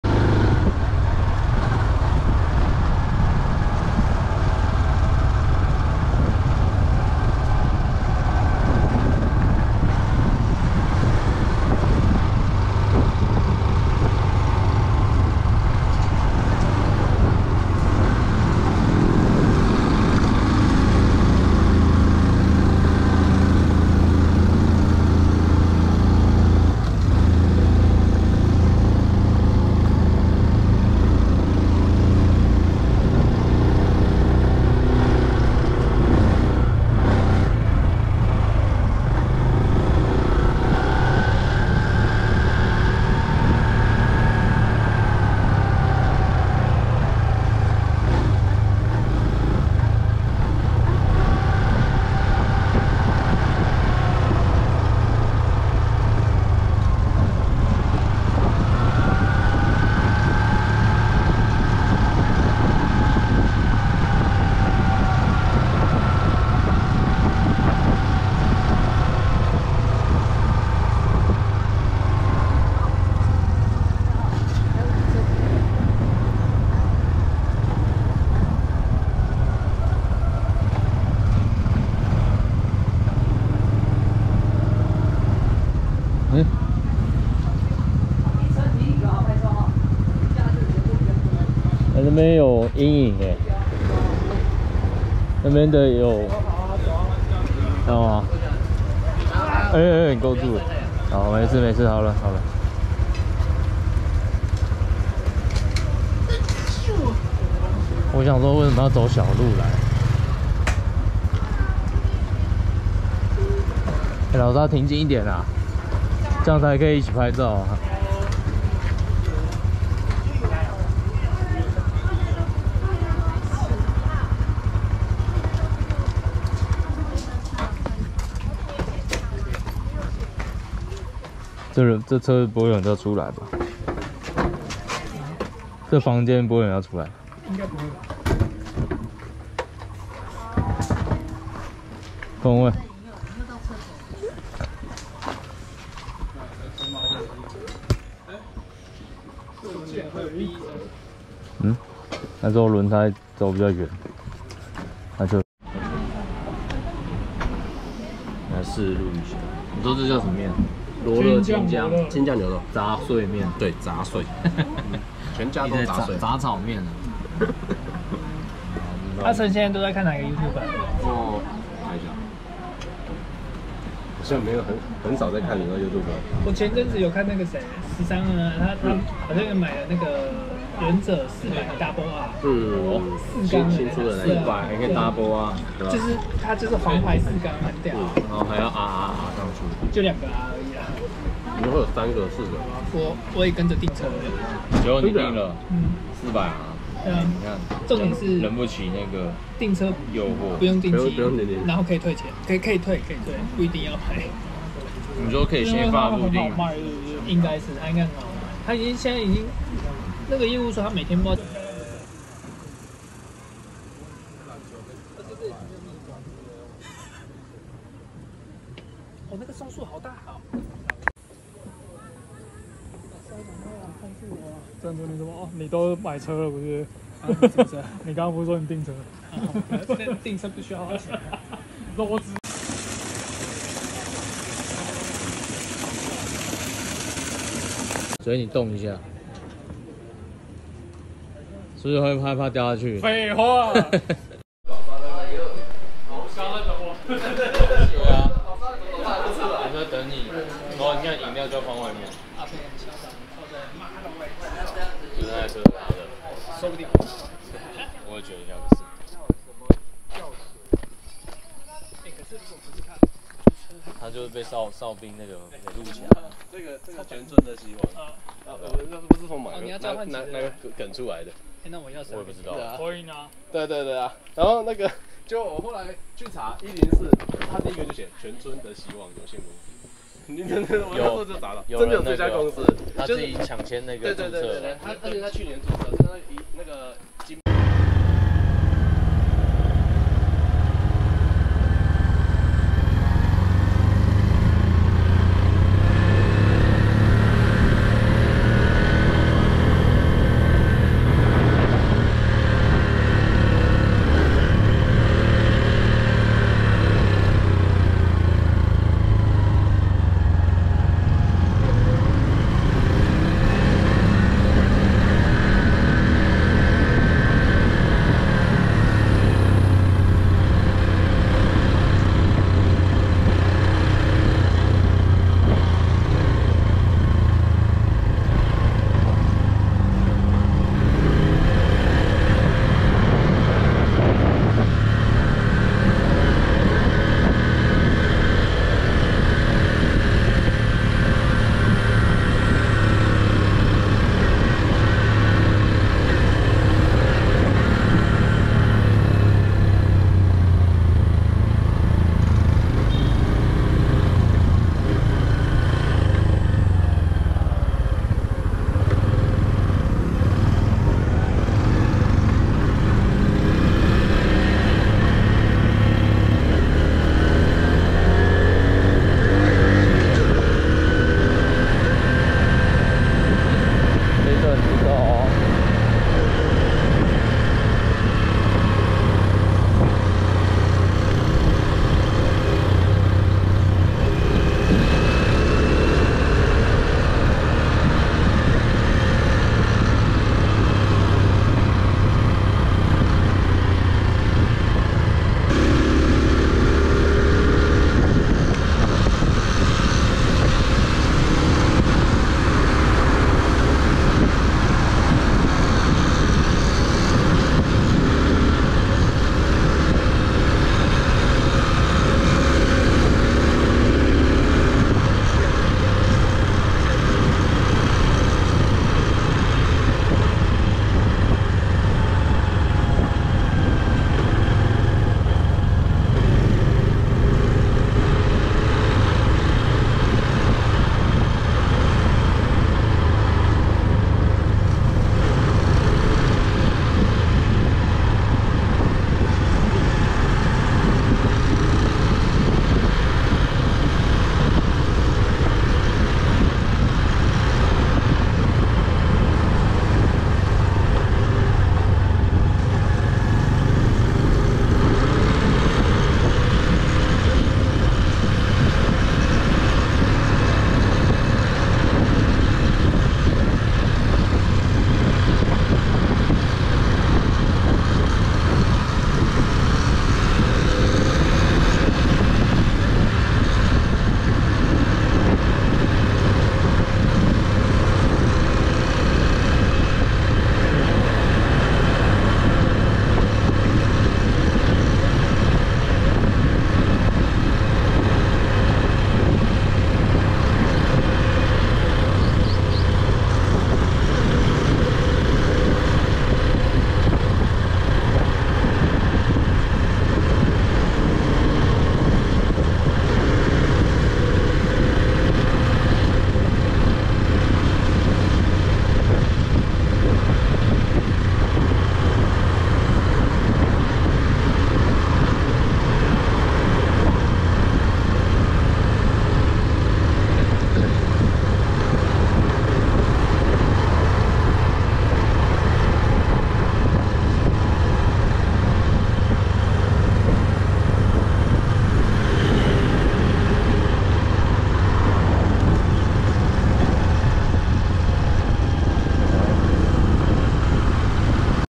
義大利東麵食館 0518| cb150r cc110 ct125 cub125 森鄰水岸
復興區農會旁冰沙喝完,
走1小段小路就會到森鄰水岸咖啡館,
這裡下午會比較多人,
中午反而不會,
建議還是訂位,才能選內或外位置。

有個小噴水池感覺能辦婚宴,
還有繩索綁的鞦韆,
感覺承重不重.